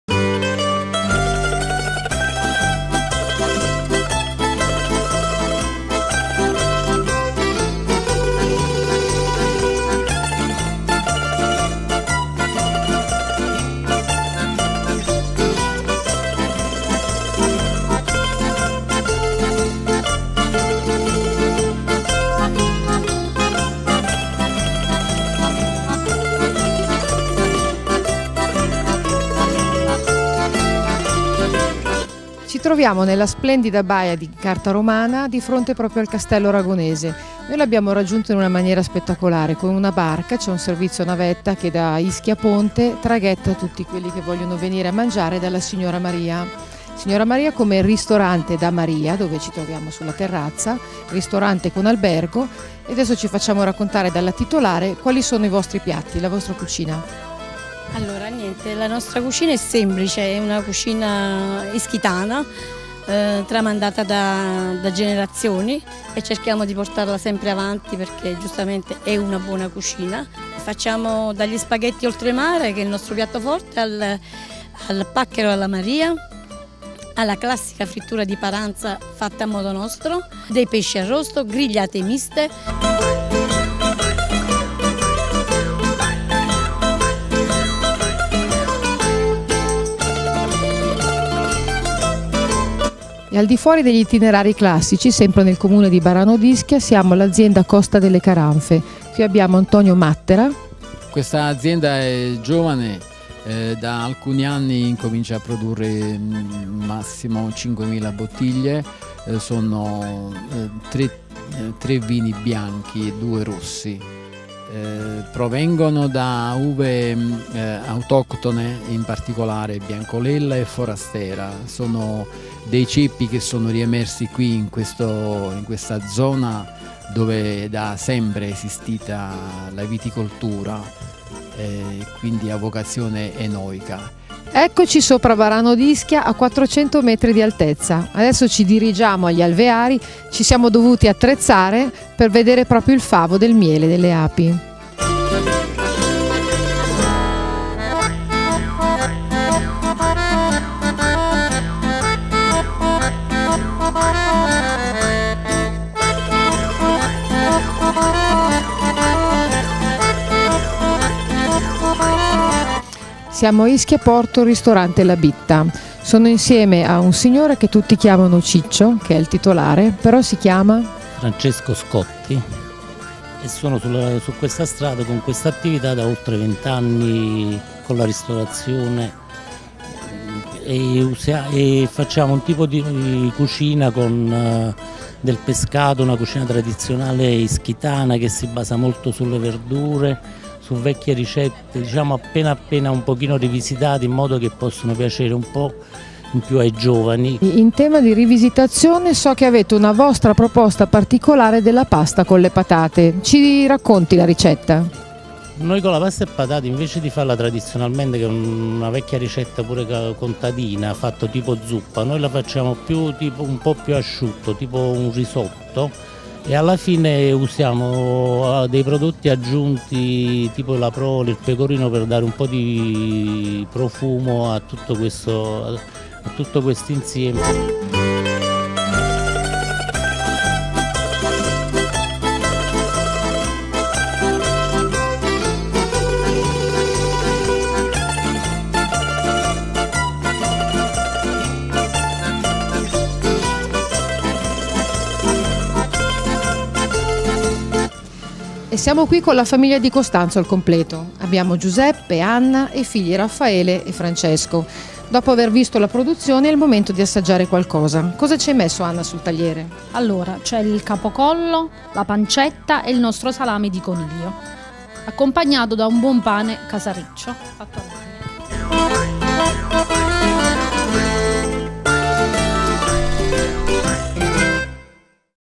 GUSTO channel : Sapori d'ischia
Attualmente il turismo è la principale risorsa economica dell'isola.
L'accoglienza è variegata, adatte sia alle famiglie che a quanti cercano una vacanza sofisticata e mondana.
La tradizione gastronomica dell'isola d'Ischia è ricca di specialità: la cucina marinara sa essere di altissimo livello e il coniglio è da sempre il piatto caratteristico. Il salame preparato con le sue carni magre è una proposta esclusiva.
La viticoltura ad Ischia ha origini millenarie.
I vigneti sull'isola si estendono dalle coste fin sugli irti pendii montani dove cellai e terrazzamenti, costruiti con rinforzi di muri di pietra a secco, consentono la coltivazione della vite